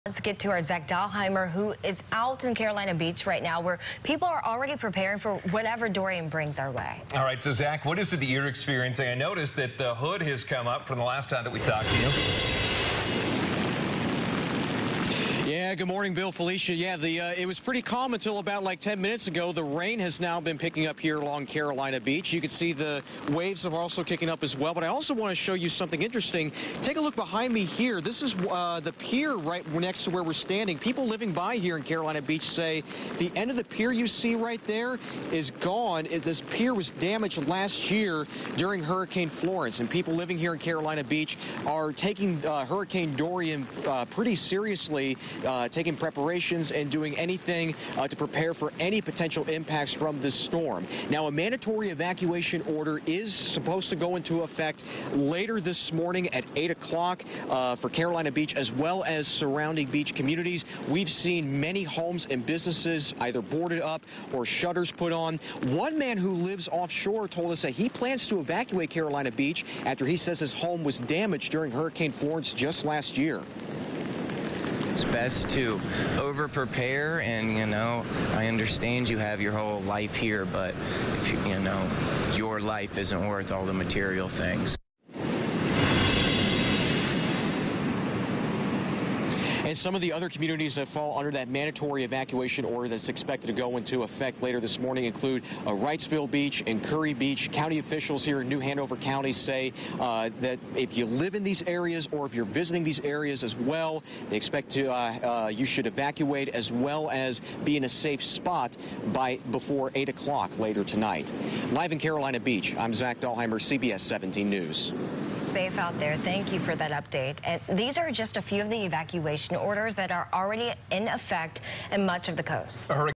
Carolina Beach to be evacuated ahead of Hurricane Dorian
A mandatory evacuation goes into effect Wednesday morning.